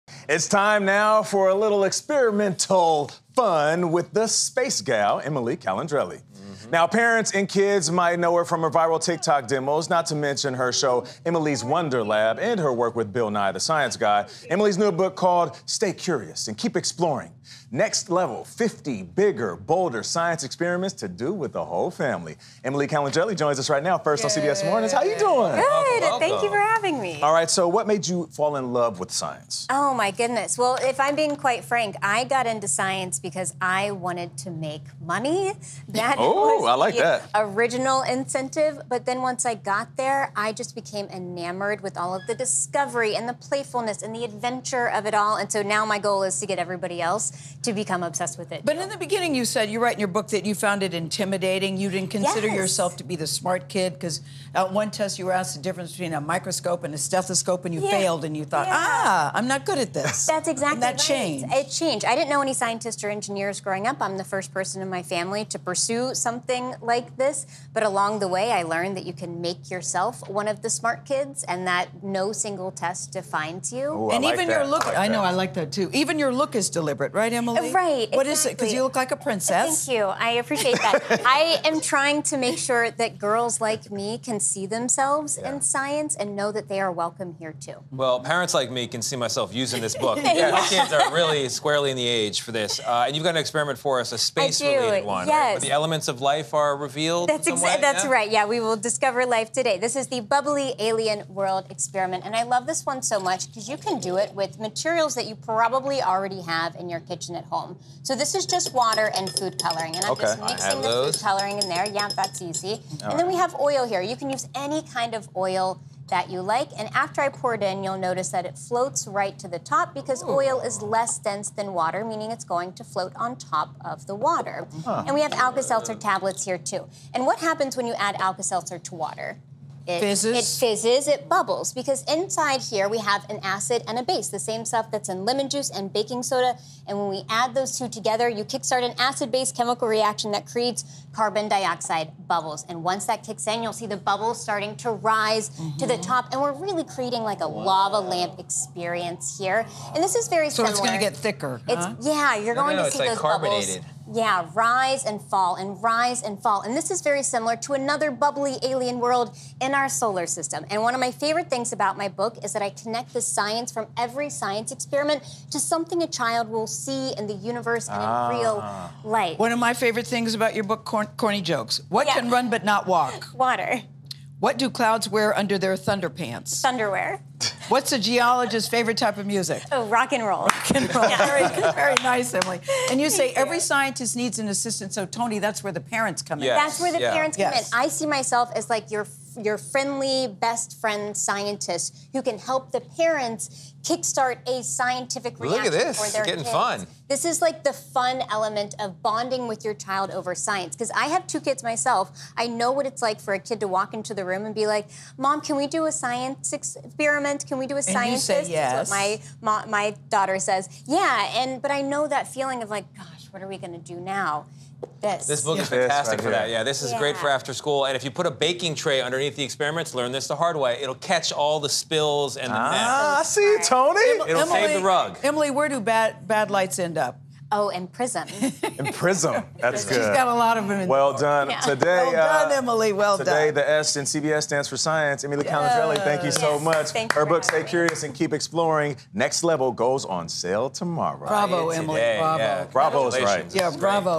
"Emily's Wonder Lab" host on new science book, collaborating with Bill Nye
Emily Calandrelli, familiar to audiences from her TikToks, Netflix show "Emily's Wonder Lab" and collaborations with Bill Nye, introduces her new book "Stay Curious and Keep Exploring: Next Level."